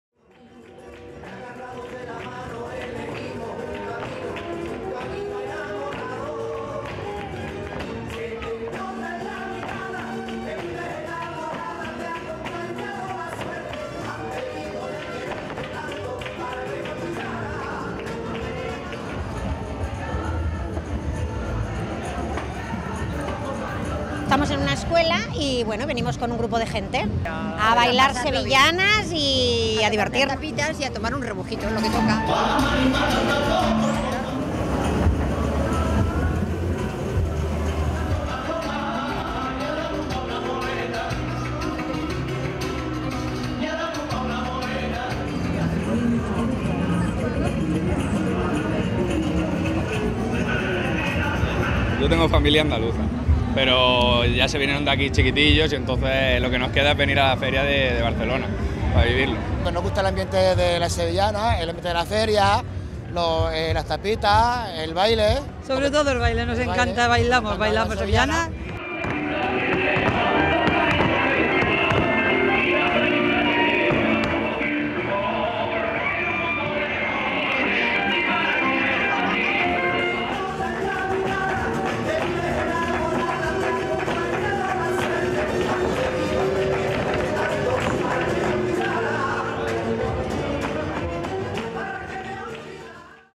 La Fira d'Abril de Catalunya arriba a la 44a edició
Sevillanes, peix fregit i bon ambient: la Fira d'Abril 2015 se celebra al Parc de Fòrum fins al 3 de maig. S'hi han instal·lat un total de 53 casetes i ocupa més de 95.000 metres quadrats.